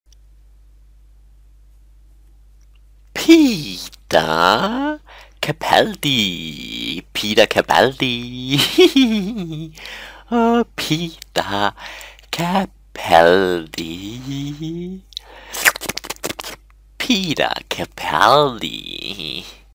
How To Pronounce Peter Capaldi
How To Pronounce Peter Capaldi is a video from PronounceGod. In this video you will learn how to say Peter Capaldi.